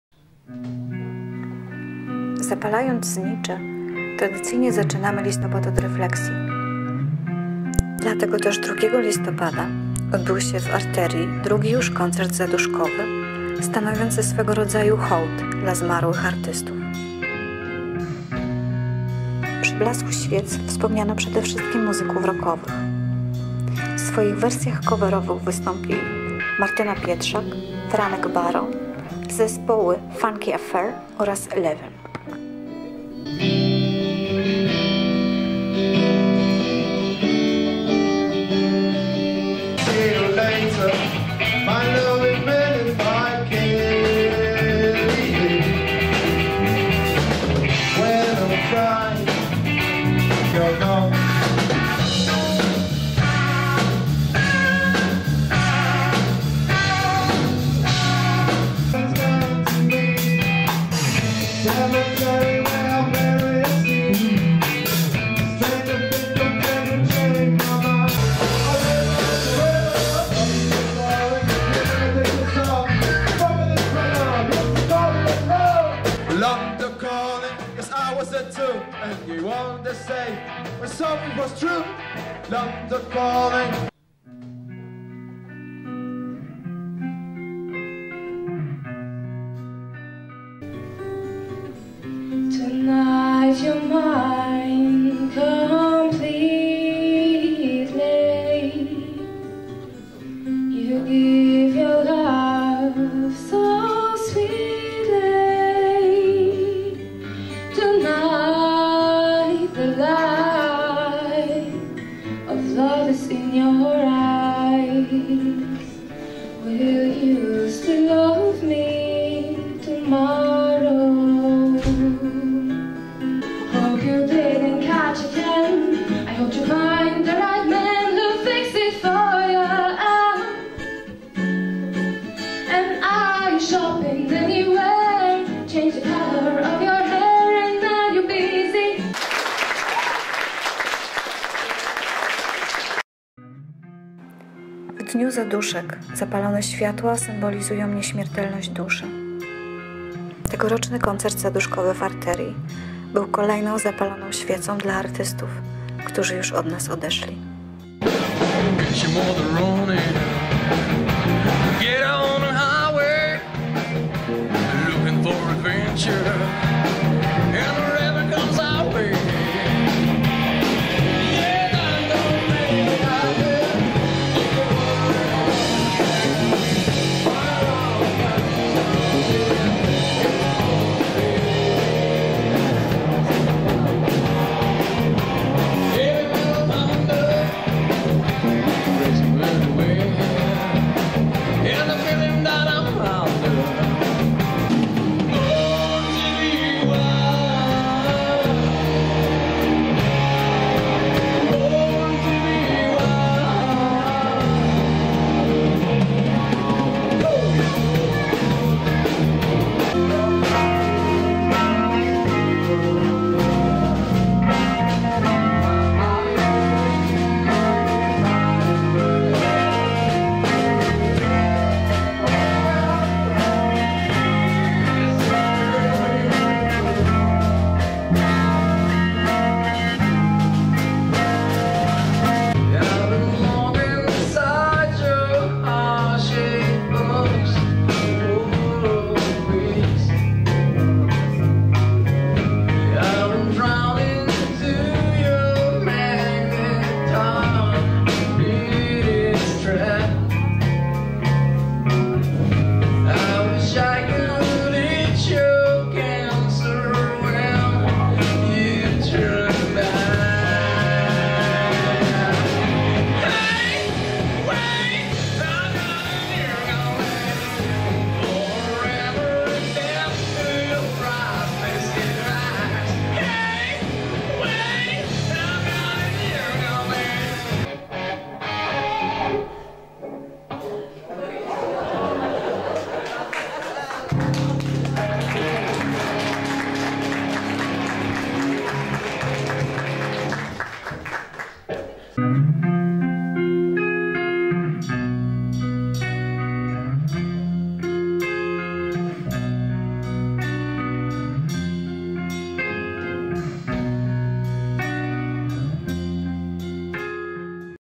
ARTERYJNE ZADUSZKI 2012
W koncercie zaduszkowym udział wzięli: Martyna Pietrzak i Maciek Zych, Franek Baron, The Funky Affair, Eleven. Zapraszamy za rok na kolejny koncert poświęcony nieżyjącym artystom.